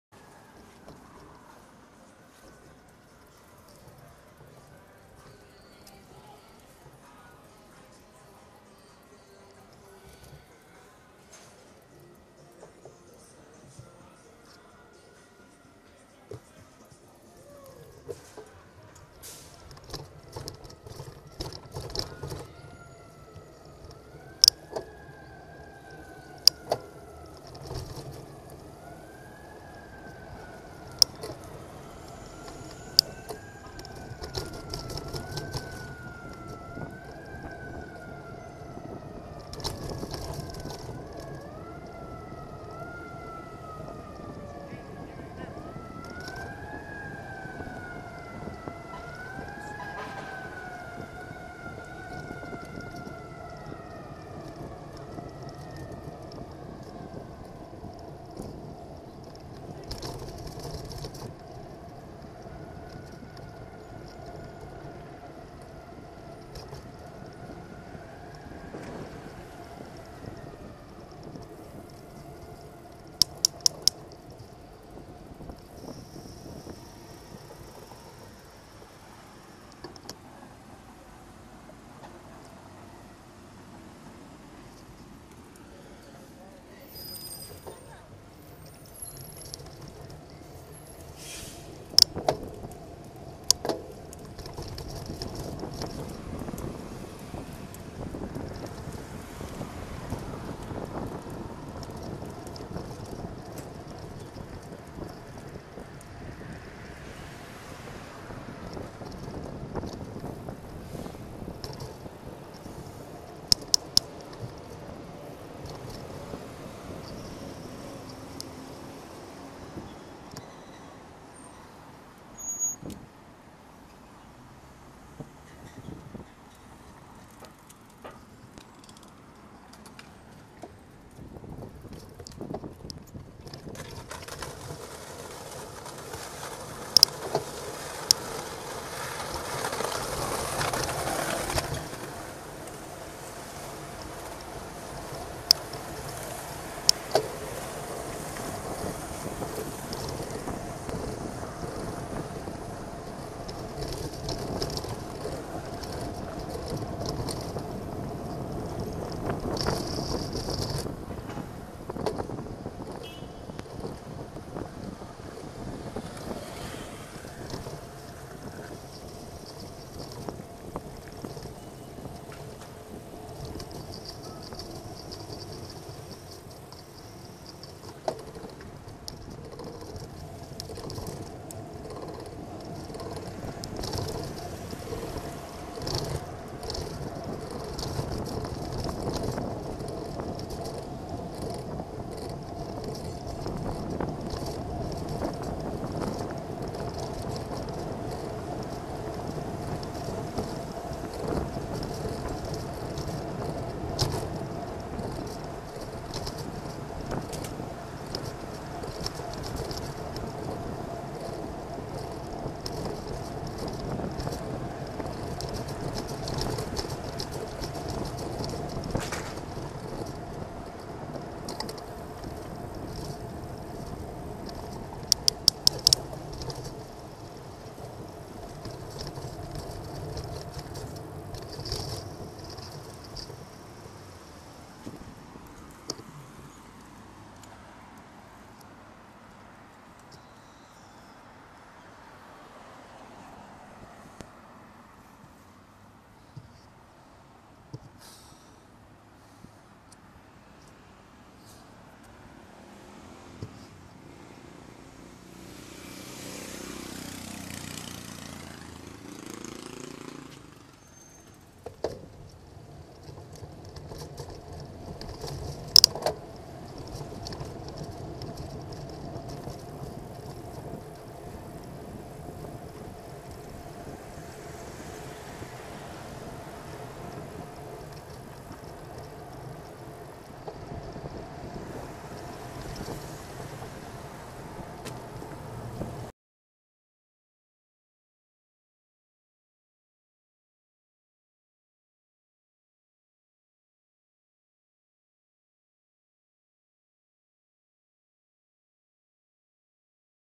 Fly 6/12 CE Picture in Picture
iMovie PiP of Cycliq Fly6 CE and Fly12 CE footage